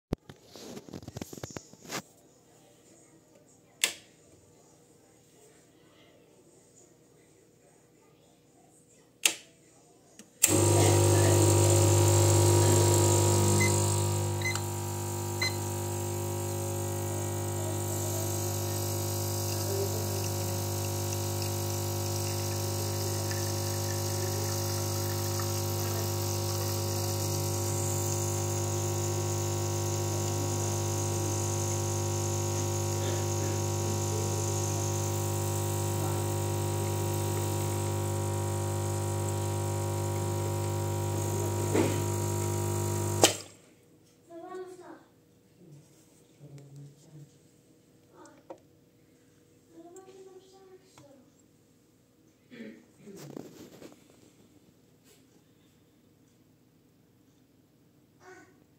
Gaggia Classic
Gaggia Classic with bottomless portafilter. 17gr in 32 out in 29 sec. Normcore hand grinder v1 at 3 clicks with medium roast coffee.